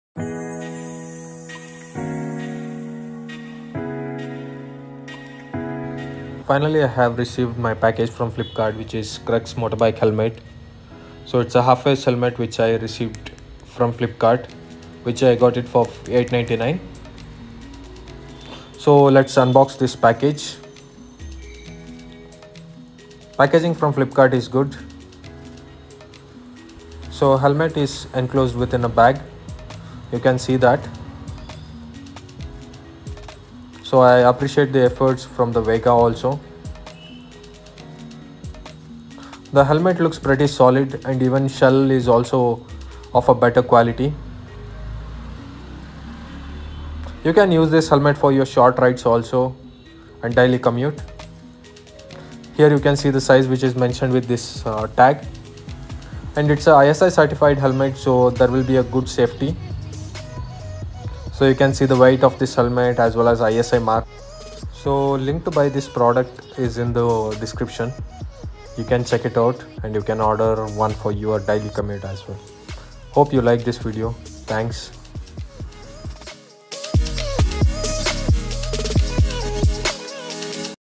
Unboxing and Review: Vega Half Face Helmet - Your Ultimate Guide to Style and Safety!
1. Vega Cliff Dx
2. Vega Cliff ISI
3. Vega Edge
4. Vega Bolt Bunny
5. Vega Crux ISI

Elevate your riding experience with the sleek and stylish Vega Half Face Helmet! 🏍️ In this in-depth unboxing and review video, we explore every detail to ensure you make an informed choice for your safety and style on the road.

👀 What's inside the box? How does it fit? What features set the Vega Half Face Helmet apart? Find out as we break it all down for you.

🛡️ Designed for maximum protection without compromising on comfort, this helmet is a must-have for every rider. From the sturdy construction to the snug fit, we've got you covered!